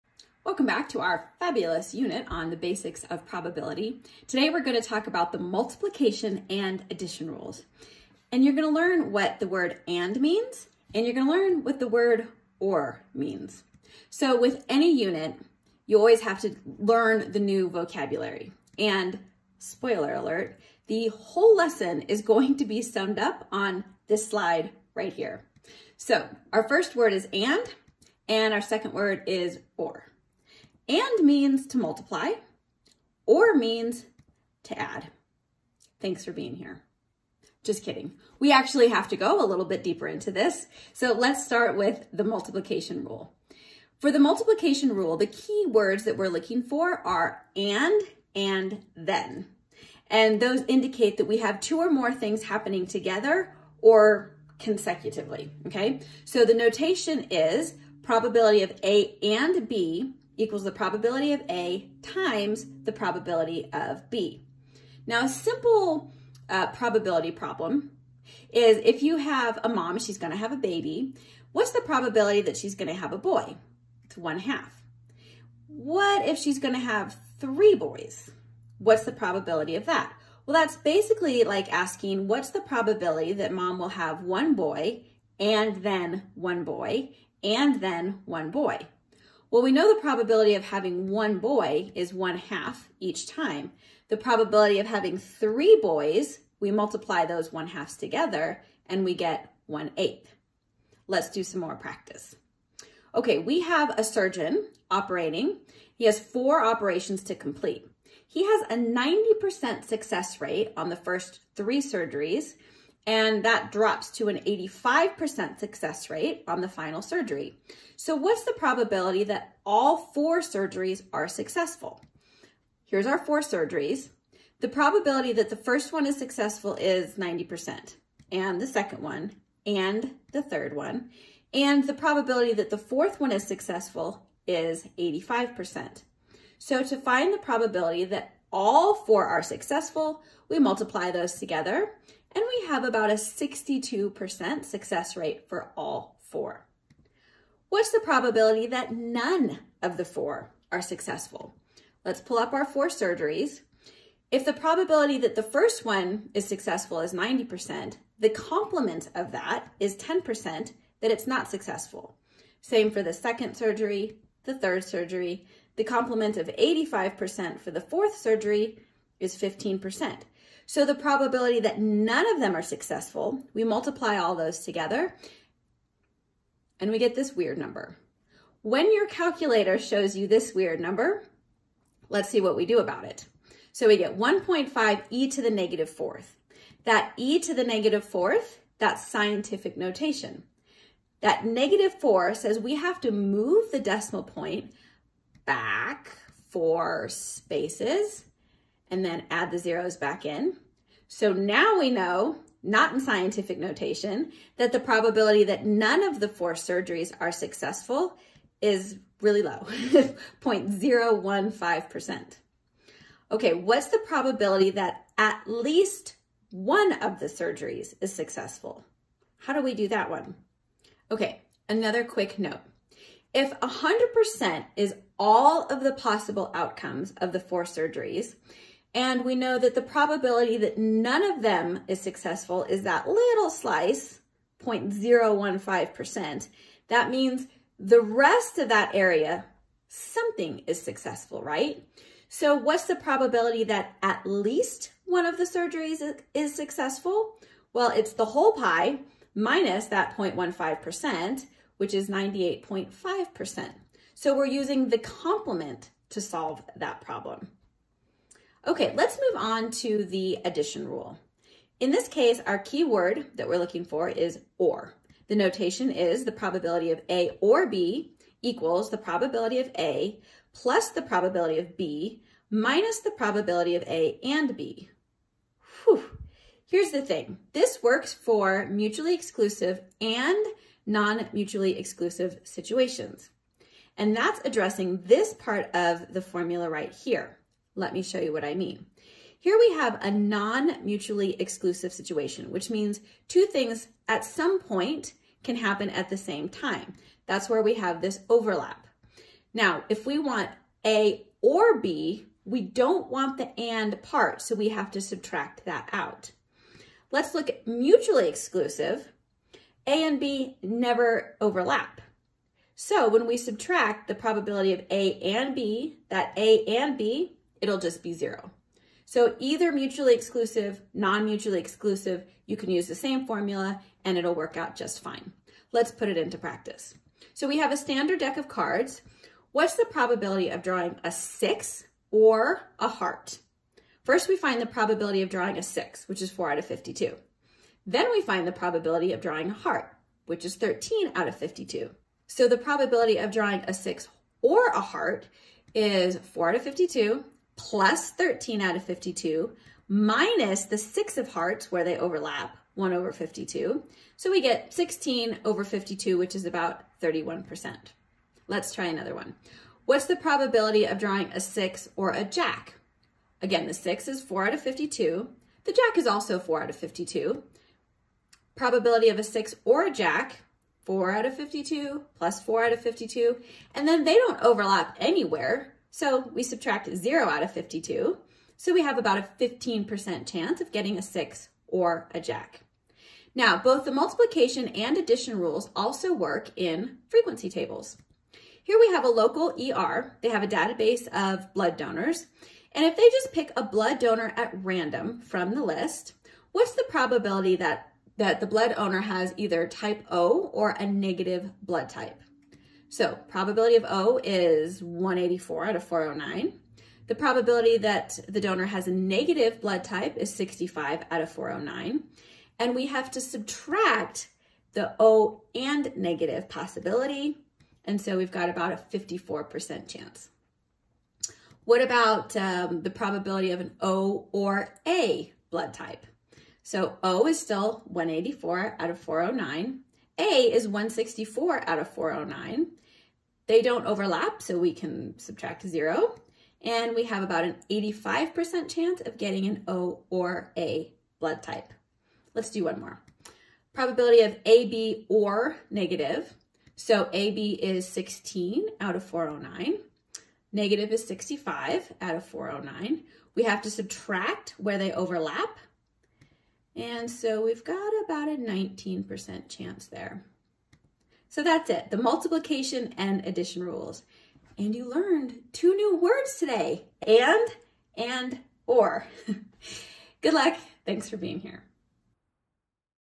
4.4 Multiplication and Addition Rule
We need to know what the words "and" and "or" mean when it comes to probability and statistics.